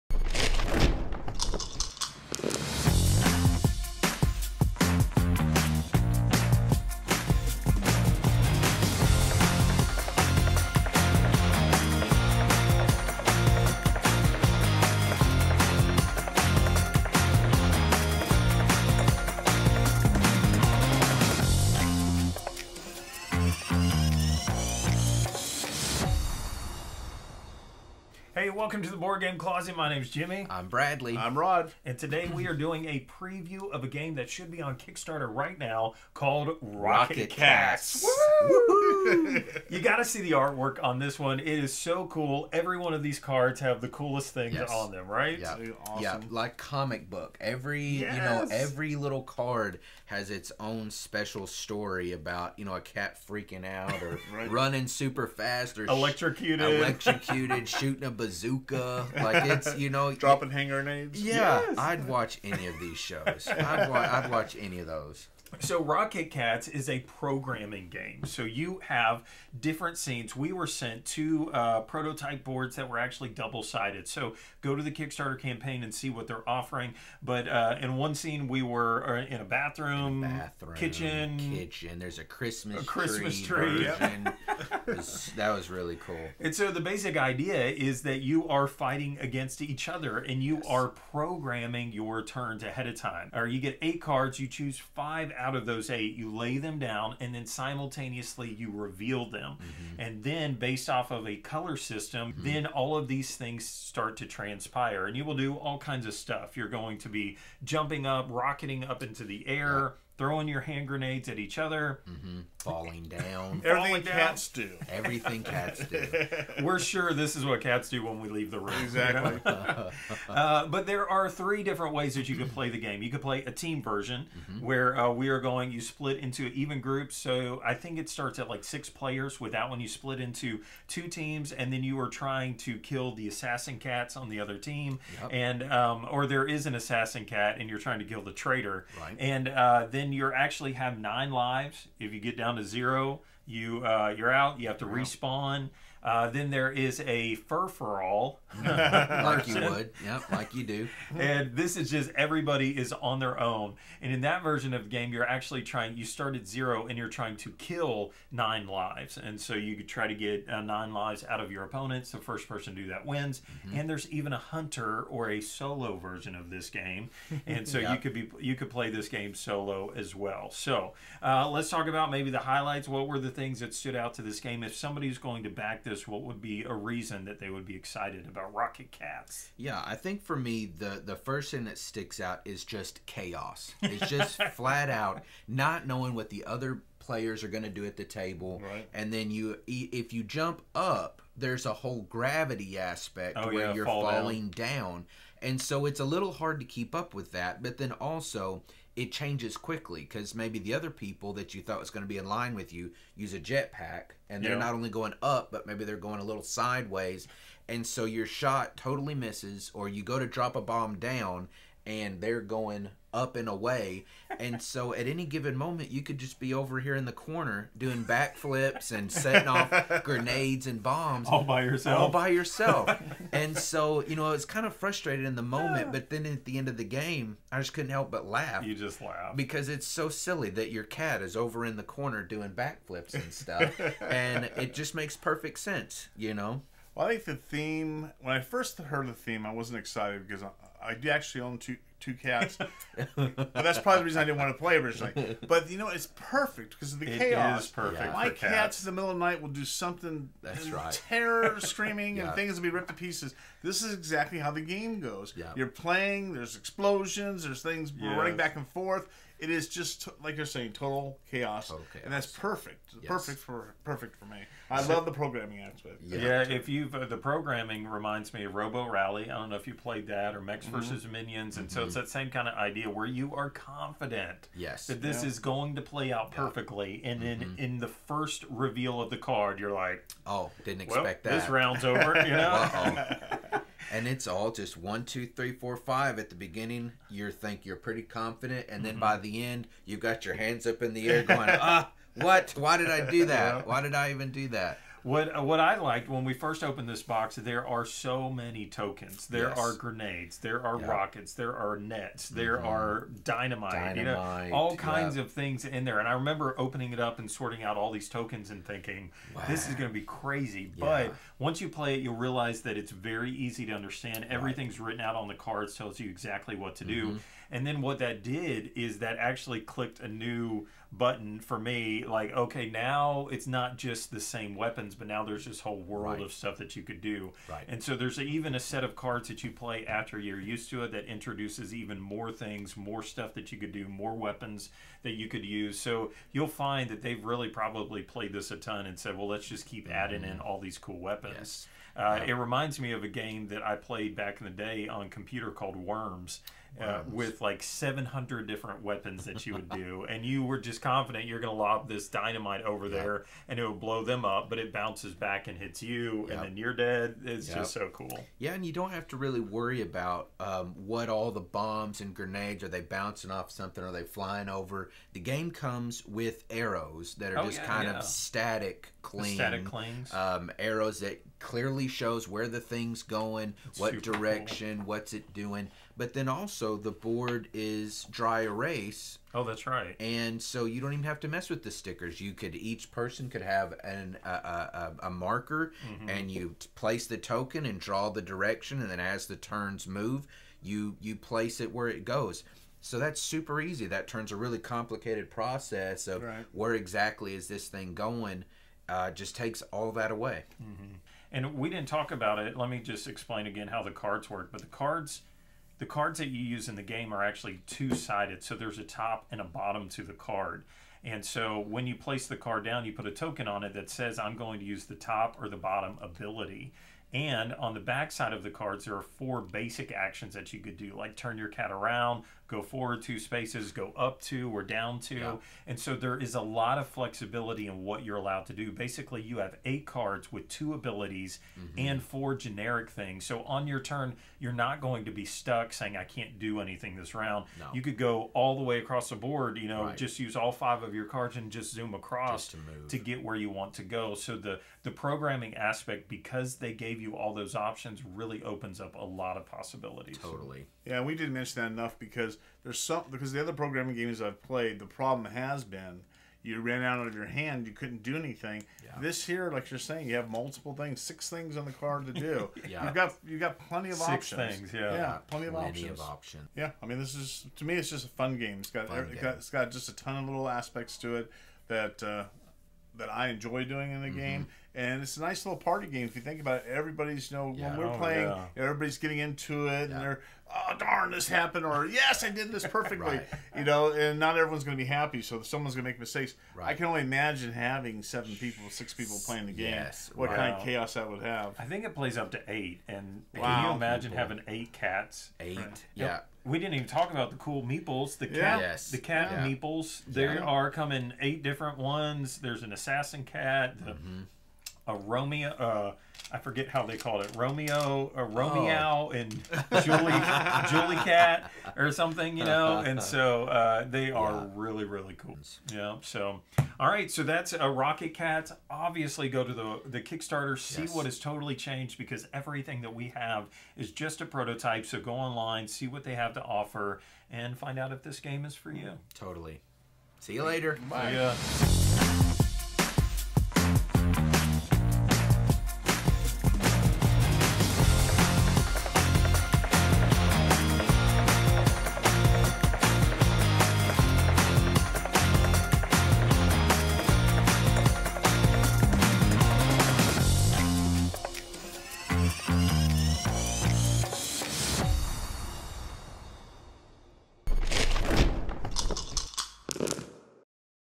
Rocket Cats Kickstarter Preview
This is a paid preview for a game live on kickstarter now.
Rocket Cats is a light-hearted, accessible, and exciting board game for 1-13 players. In the main game (Team mode), your family of cats is try to take out your traitorous family member (Romeow if playing as the blue family, or Julicat if playing as the pink family). The first team to take all 9 lives of their traitor wins the game.